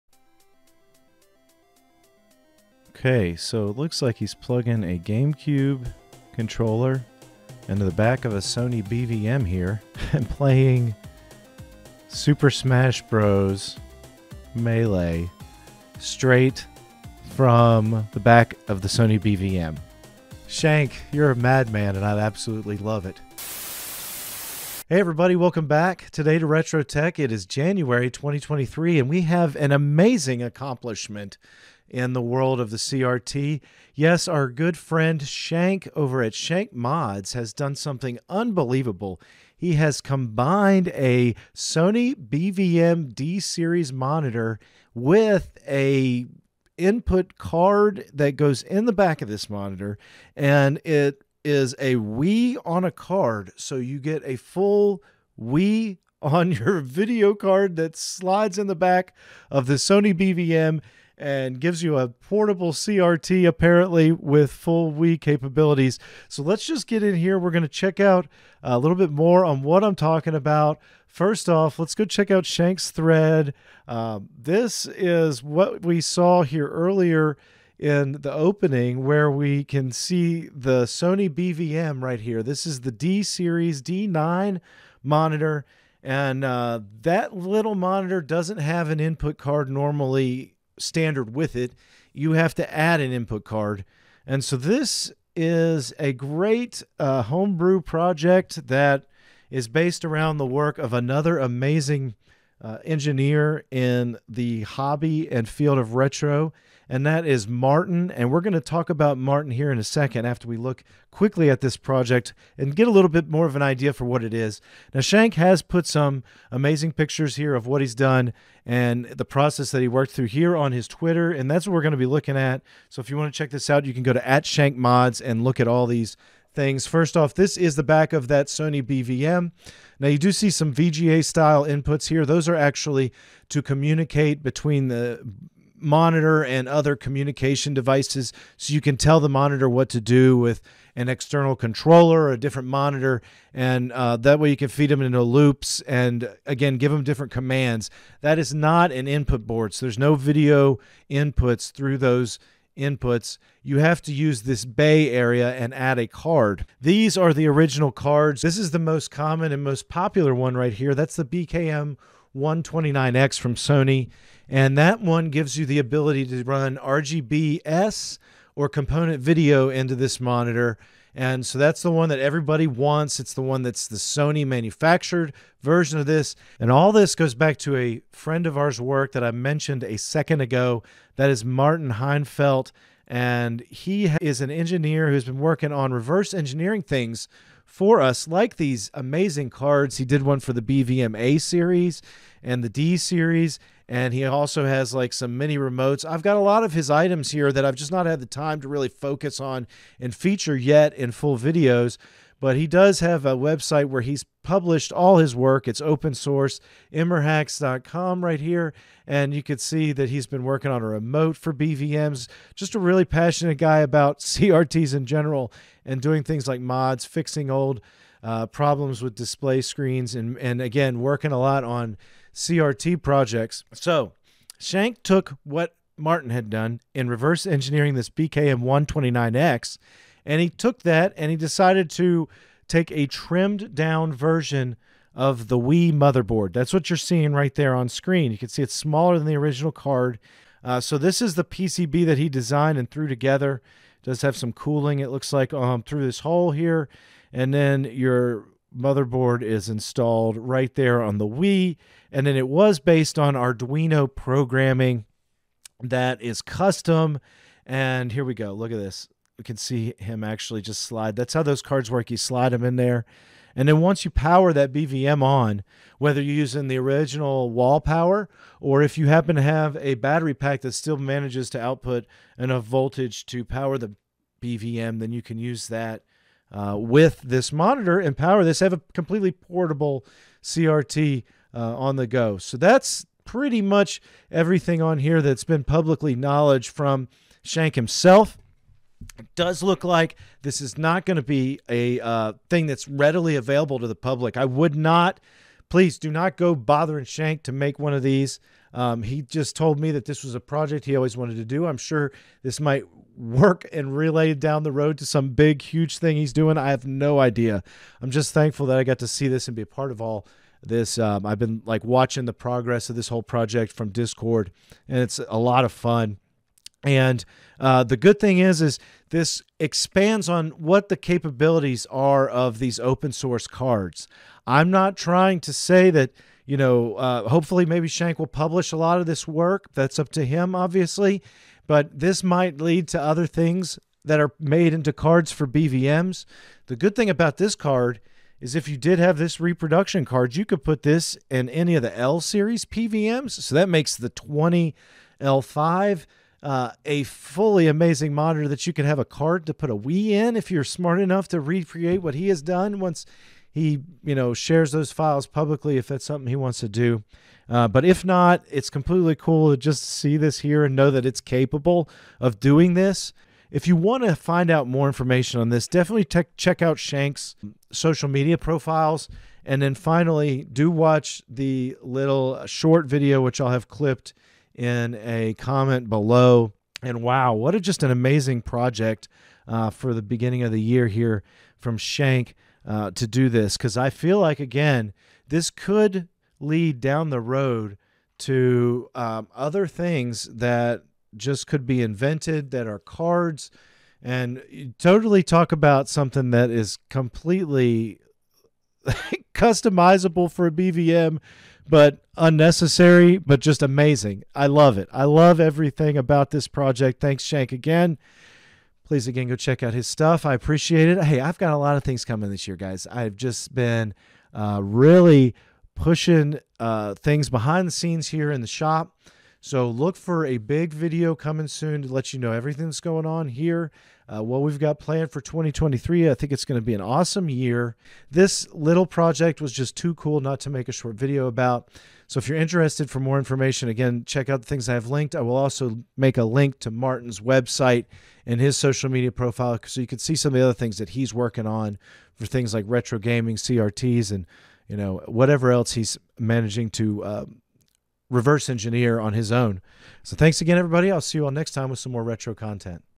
A Wii installed inside a CRT?!
@ShankMods has done the unthinkable.  He has created an external input card for Sony BVM CRT monitors that is a Wii!  The Sony BVM is a D9H5U CRT.